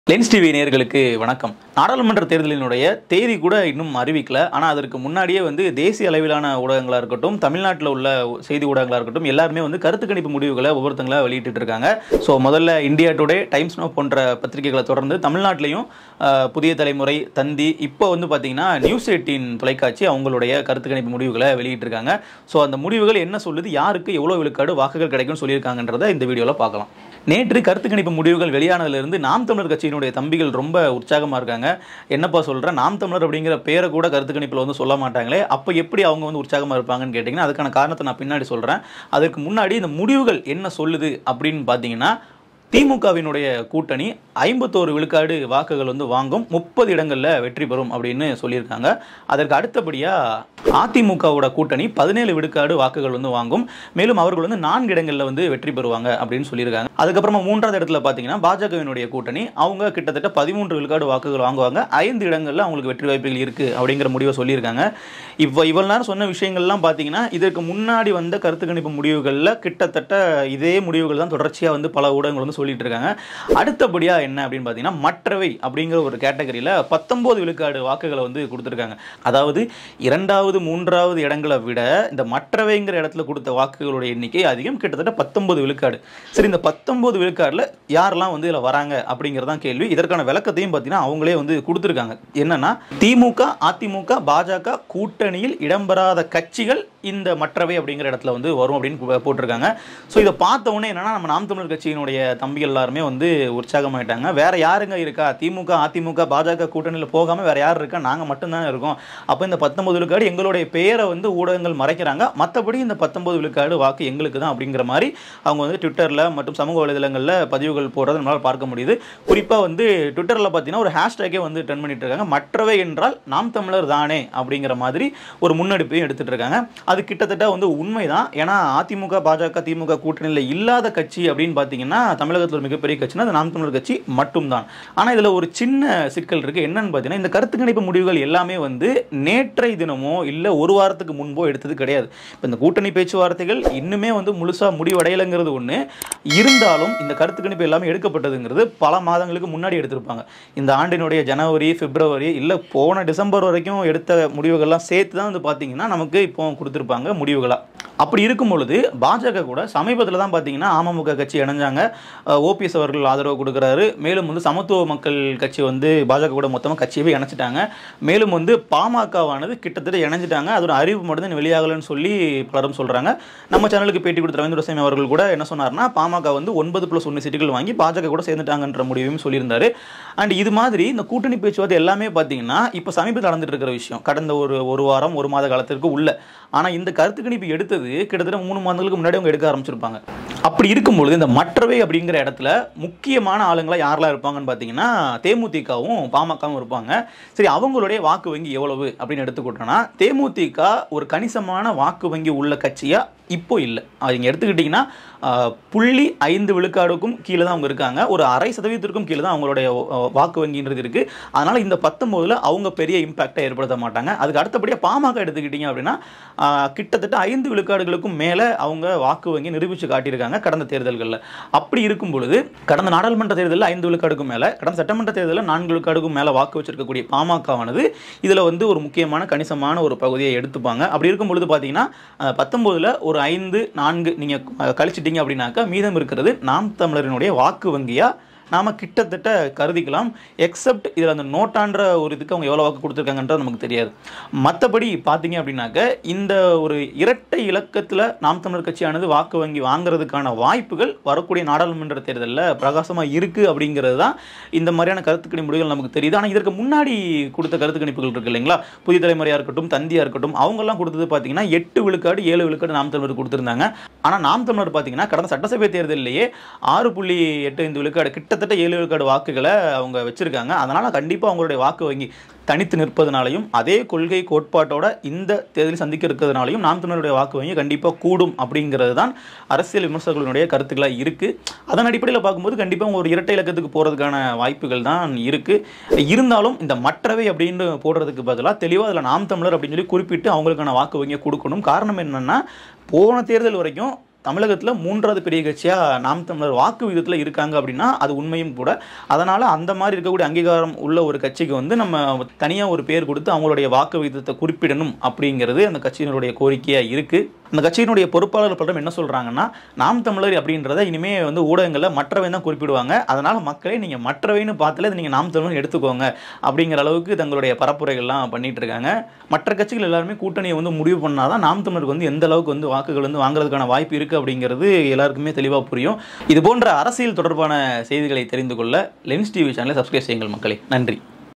2024 நாடாளுமன்ற தேர்தலில் நாம் தமிழருக்கு எத்தனை சதவீதம் வாக்கு? | Survey results
நாடாளுமன்ற தேர்தல் கருத்துக்கணிப்பு முடிவில் நாம் தமிழர் கட்சியை மற்றவை என குறிப்பிட்டு 19% வாக்குகளை வழங்கிய நியூஸ் 18 தொலைக்காட்சி..!